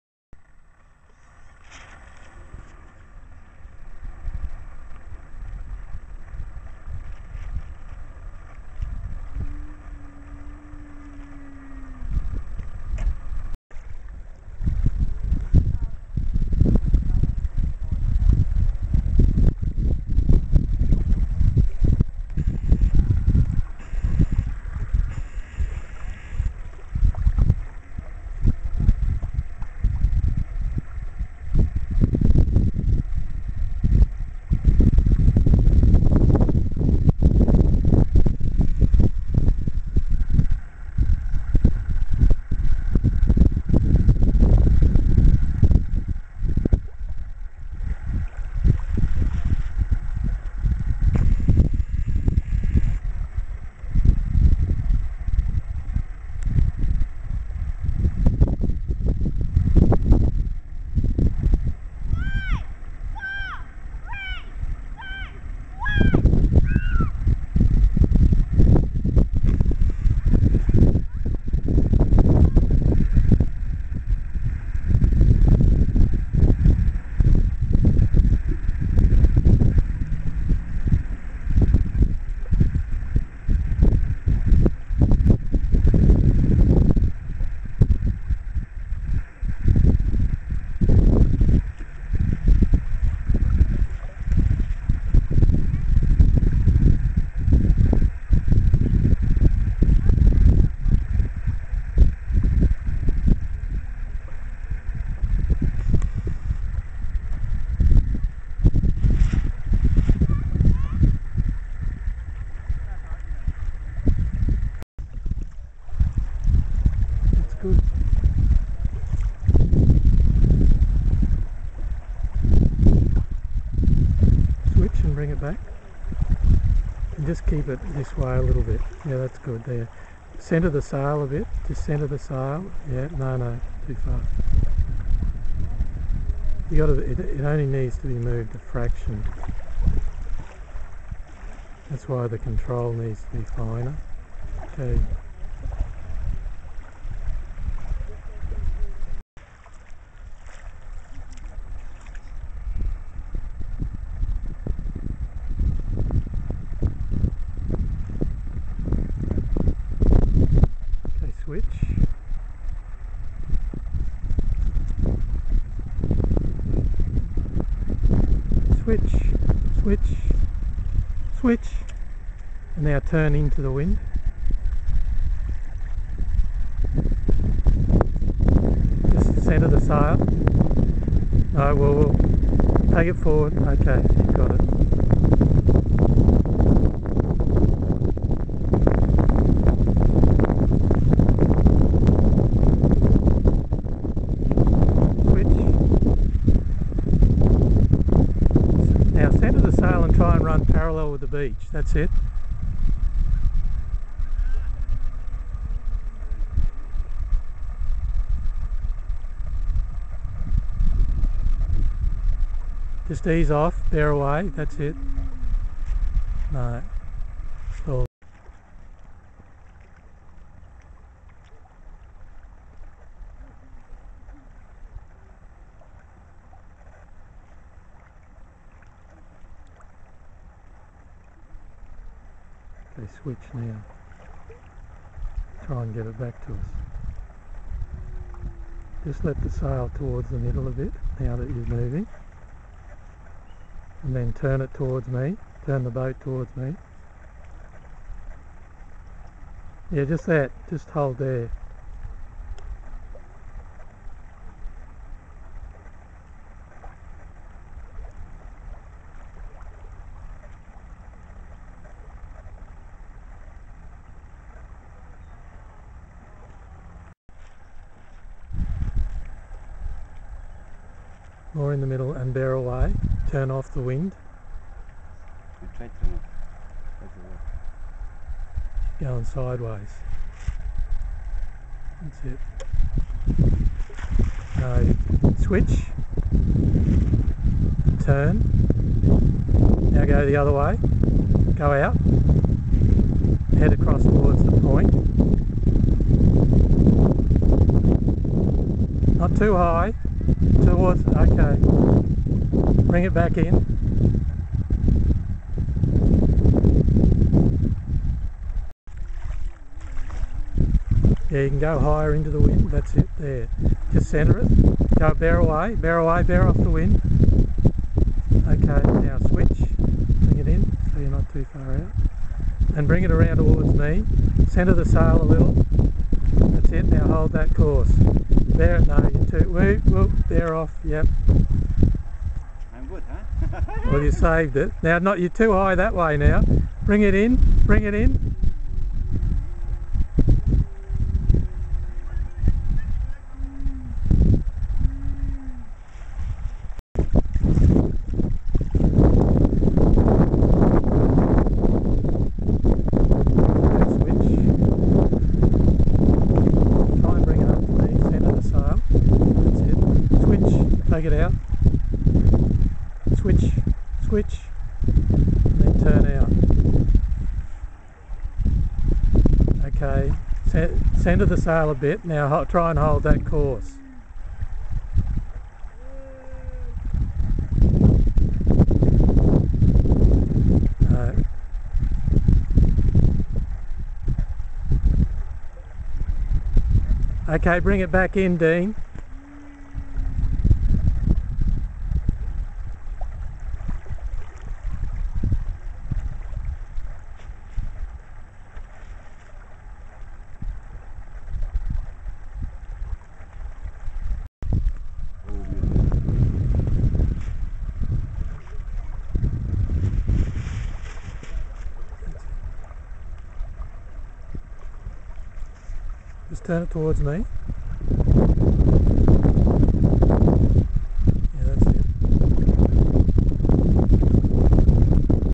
Dean_Model_Proa
Quarter scale model of sailing proa having bi-directional canted sail.